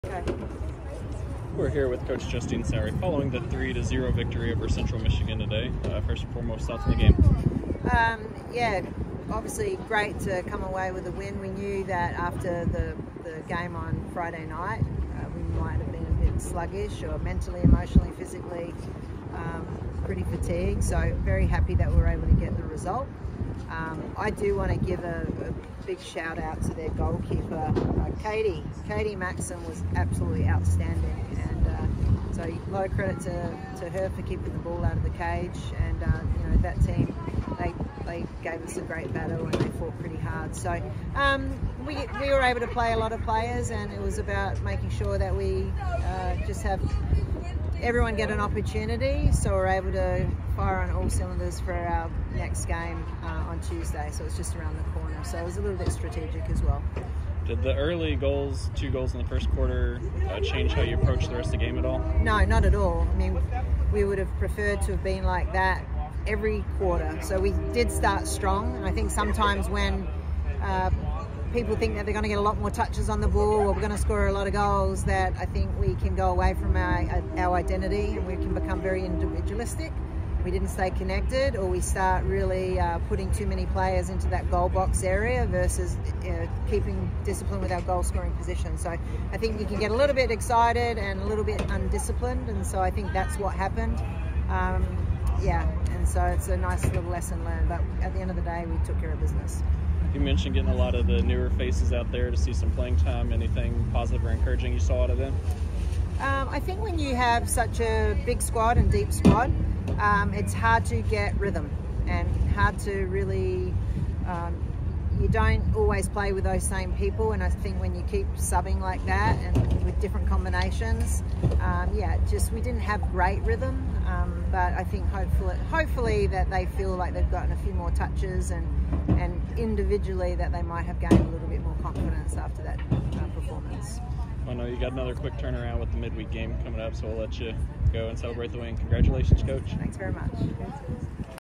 FH: Coach Justine Sowry Postgame Interview vs. Central Michigan
Louisville head coach Justine Sowry recaps the 3-0 victory over Central Michigan.